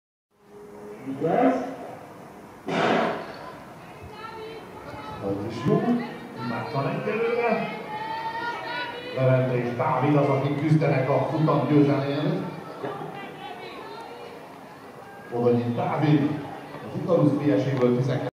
80 m-es gátfutás újonc fiú 2. ief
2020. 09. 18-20. Tatabánya, Magyarország Újonc és Serdülő Atlétikai Bajnoksága